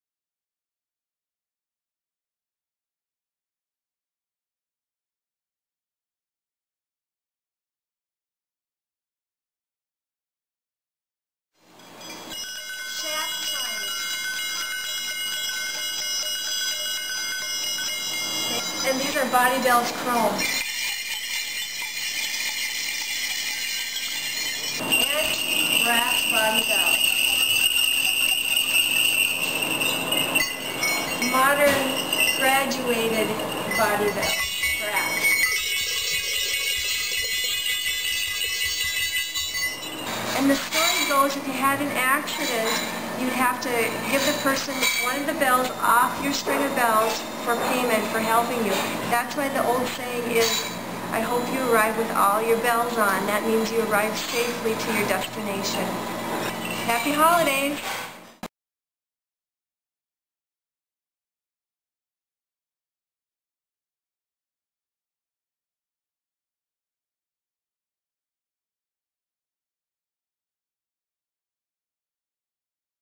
Sleigh Bells with Bobbie Kleffman
Bobbie Kleffman, All Things Equine host on 91.7 KAXE, brought in a selection of sleigh bells to the studios and shared some stories and information about the sounds and history of bells.